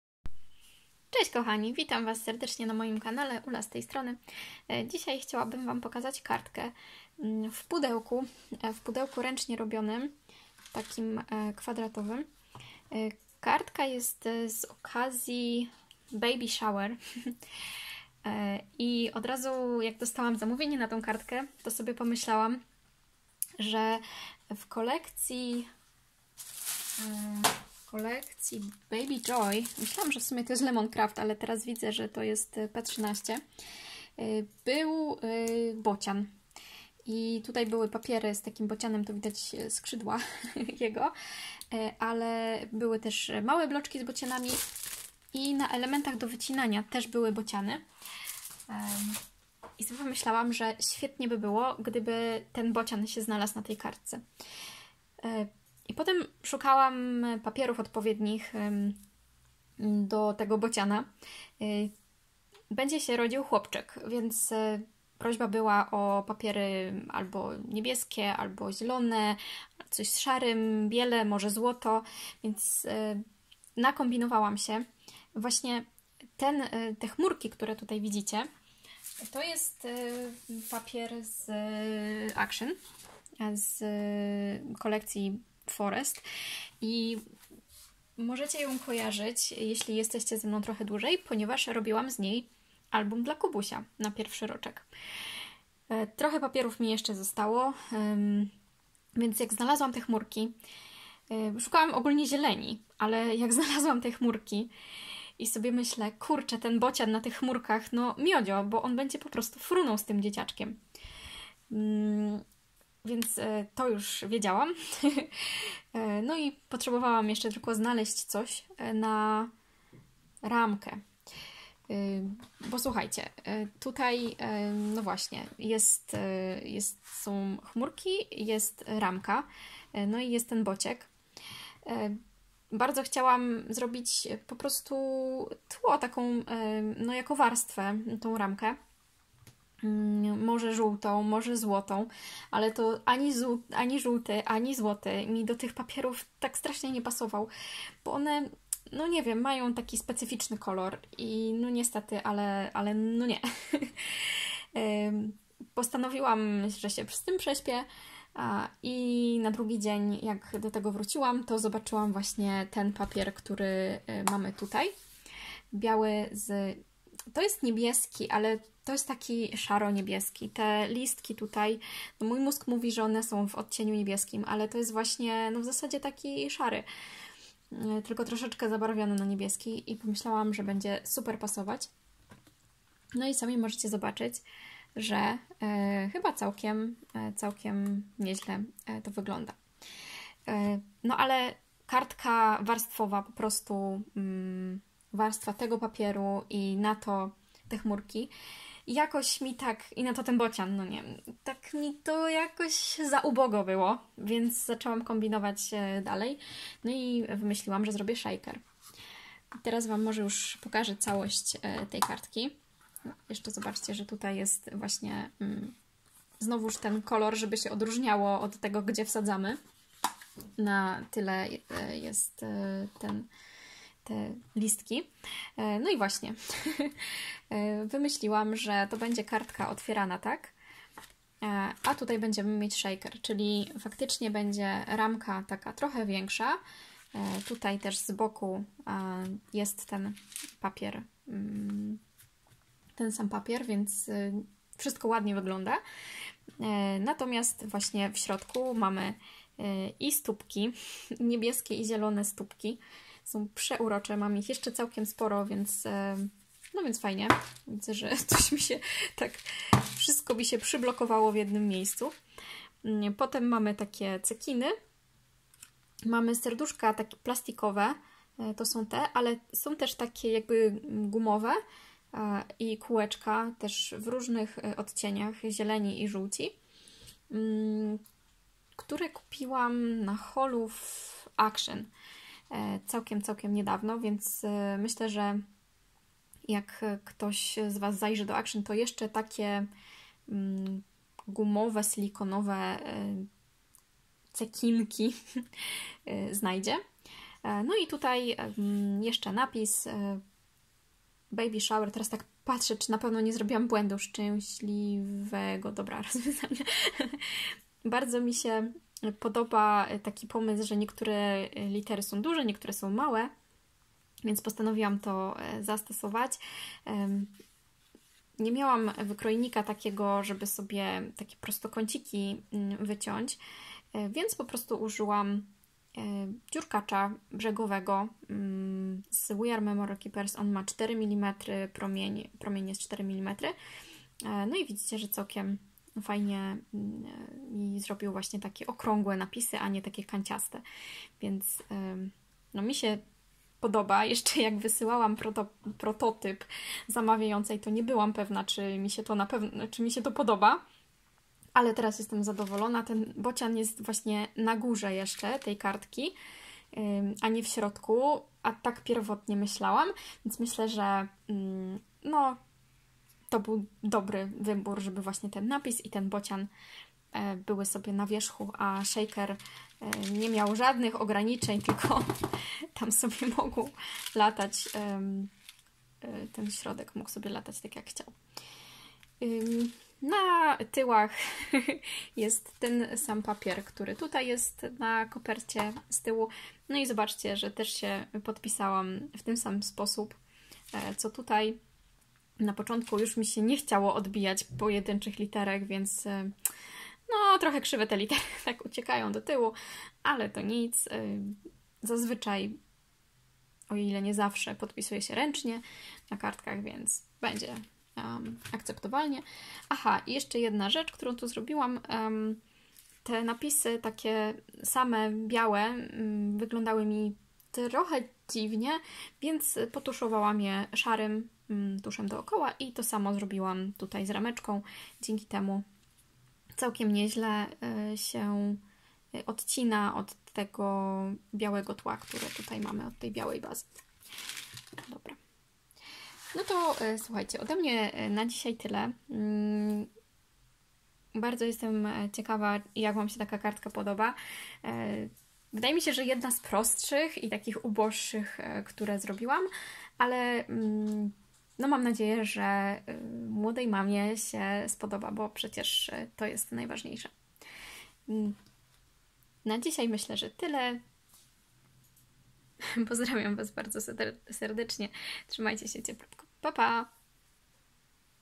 Kartka dla przyszłej mamy | Baby shower card
PL: Kiedy przyjaciółka pyta czy poratuję ręcznie robioną kartką, nie zastanawiam się długo - sprawdzam tylko termin realizacji ;) Co prawda okazało się, że pomieszały mi się miesiące i na zrobienie kartki miałam ponad miesiąc, ale cóż... w ten sposób powstała w kilka dni ;)
A kartka jest z nie byle jakiej okazji, bo na Baby Shower. Od razu do głowy przyszedł mi bocian z kolekcji Baby Joy od P13. A co do niego? W bloszku Forest z Action znalazłam idealnie pasujący papier w chmurki. Do tego trochę cekinów w shakerze, najlepsze życzenia i mamy to :)

Bociana z tej kolekcji użyłam również w kartce z okazji zajścia w ciążę a album dla Kubusia znajdziecie tutaj

EN: When a friend asks if I'll save her with a handmade card, I don't think long - I just check the deadline ;) Admittedly, it turned out that I got the months mixed up and I had more than a month to make the card, but well... this way it was created in a few days ;)
And the card is for not any occasion, but for a Baby Shower. Immediately to my mind came the stork from the Baby Joy collection from P13. And what to add to him? I found a perfectly matching cloud paper in the Forest block from Action. Plus some sequins in a shaker, stamped text with best wishes for childbirth and we have it :)